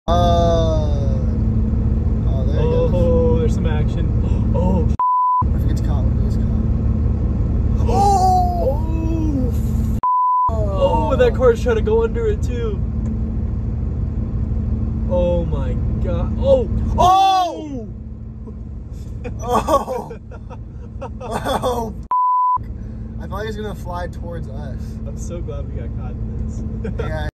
Car tries to pass under downed powerline on Texas highway
Viewer Hank Lehning shared video while pulled over on the freeway, showing a downed powerline across Interstate 35 north of Austin by Round Rock.